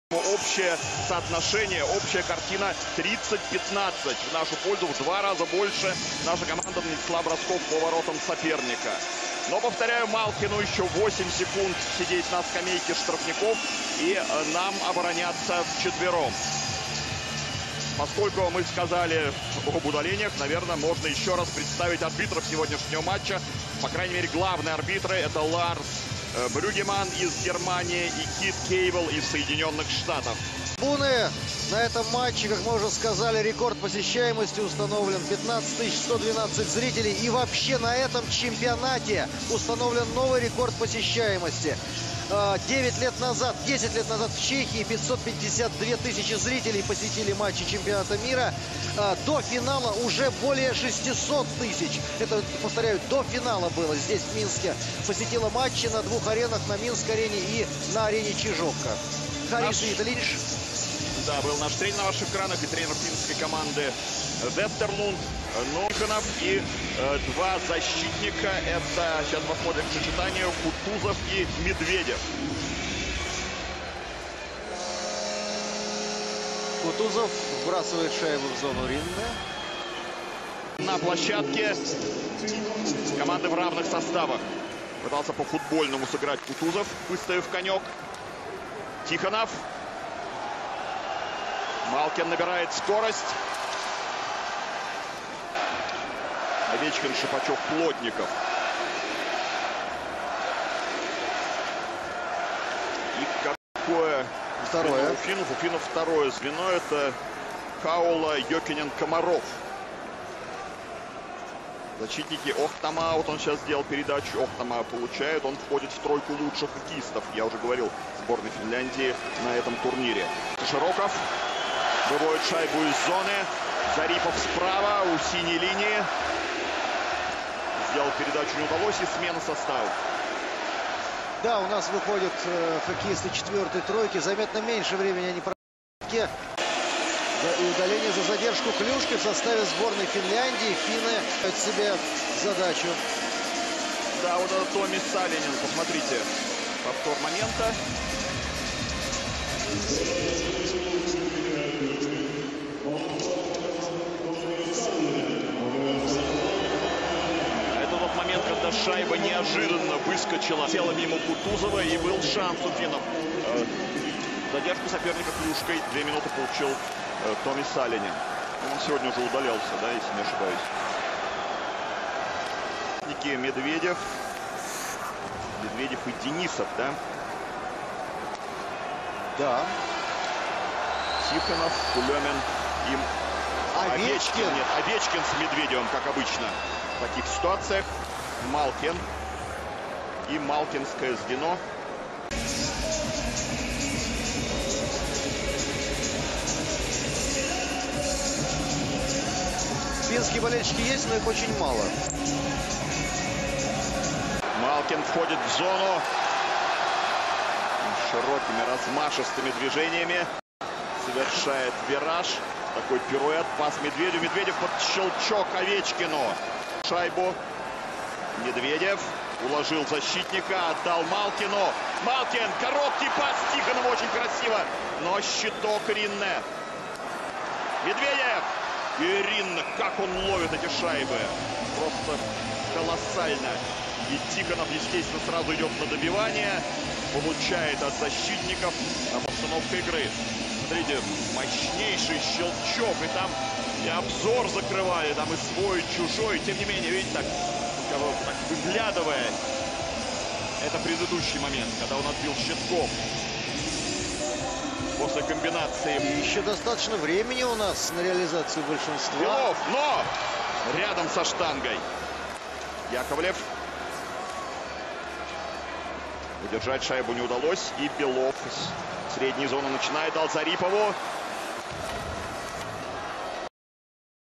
Хоккей Чемпионат мира 2014 Россия Финляндия финал 3period(1)
Хоккей Чемпионат мира 2014 Россия Финляндия. Лучшие моменты игры: броски, сэйвы, драка, удары